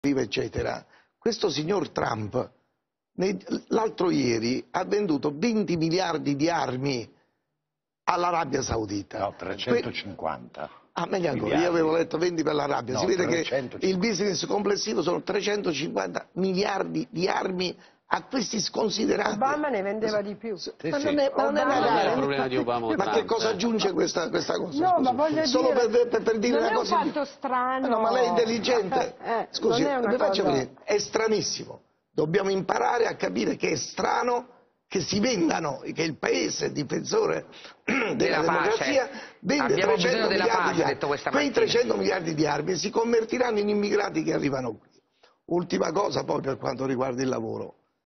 De Masi: 'Trump vende armi, che si convertono in immigrazione'
Il commento alla visita a Roma del presidente USA Donald Trump dopo l'incontro con Papa Francesco